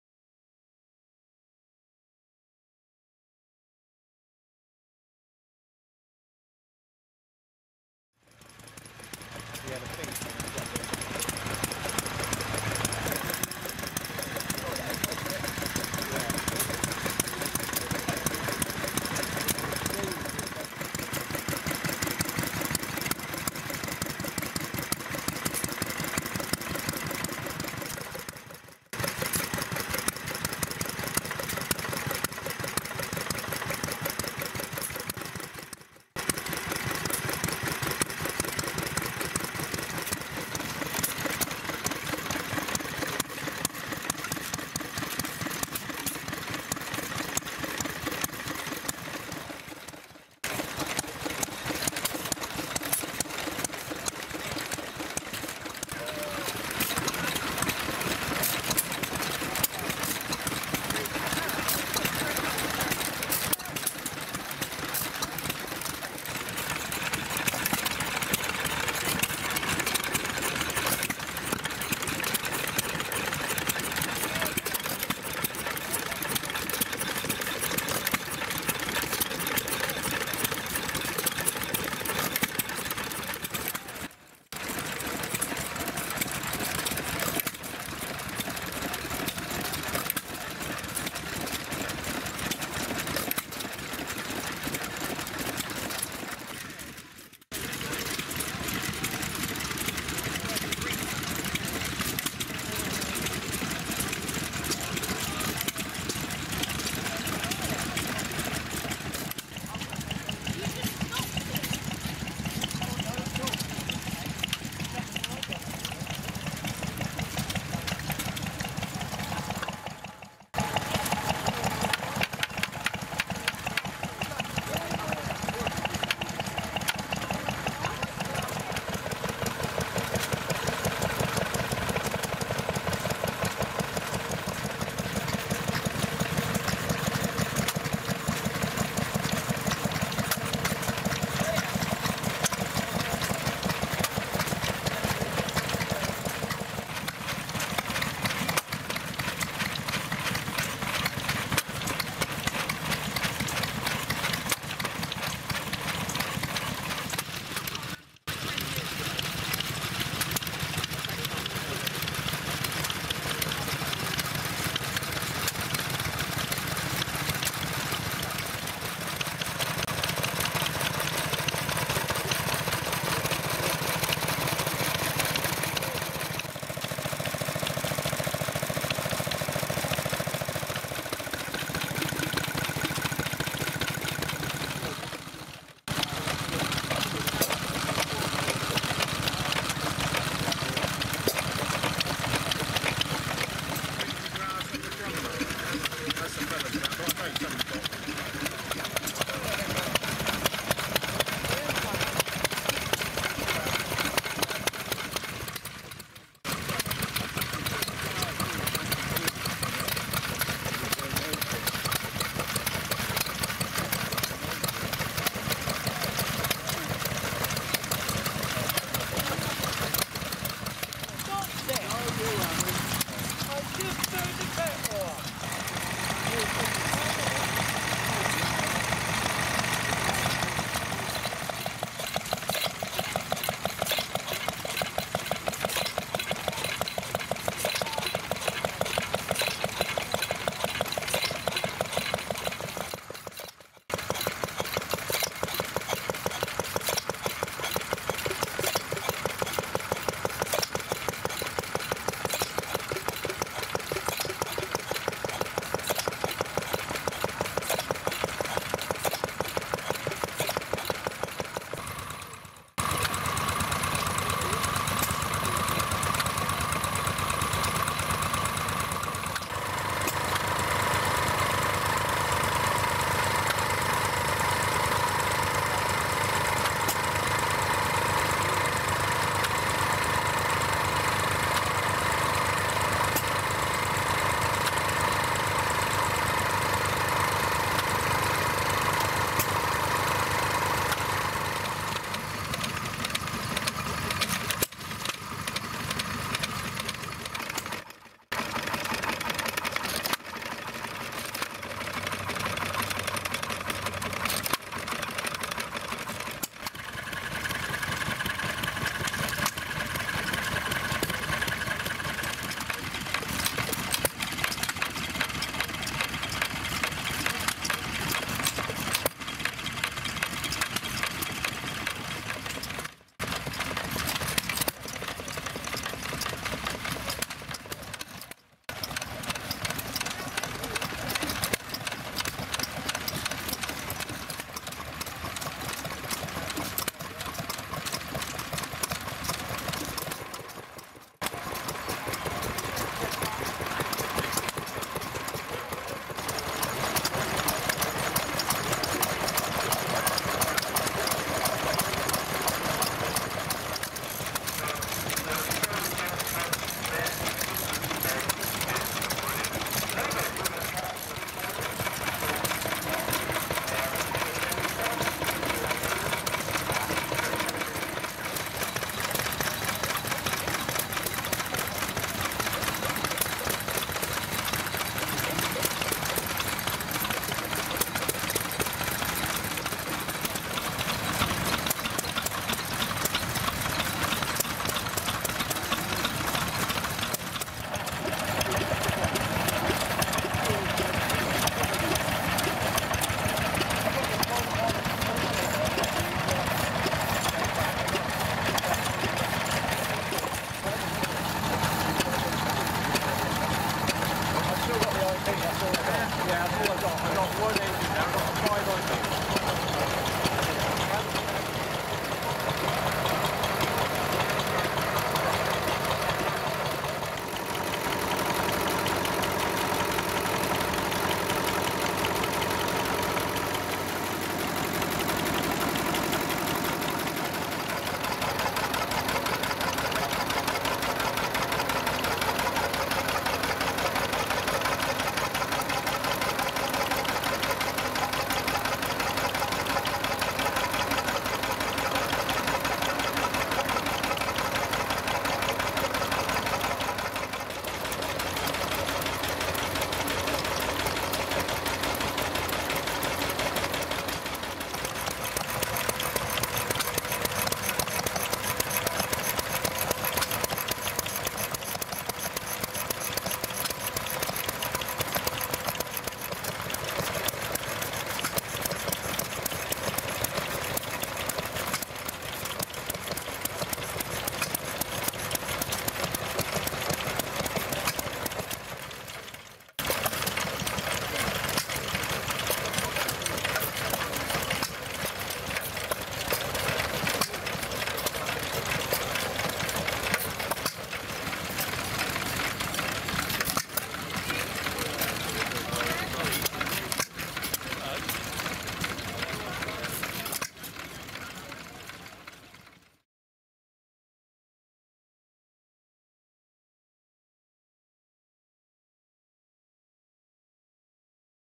The Stationary Engines section at the Longstock Vintage Rally. Danebury. Andover. 12.09.2021.
There were many of these Stationary Engines at the Rally, and I've tried to show them all, but with a short viewing of each and not lingering for a long time, otherwise it'll make the video too long. Met some regulars from other shows I've been to too.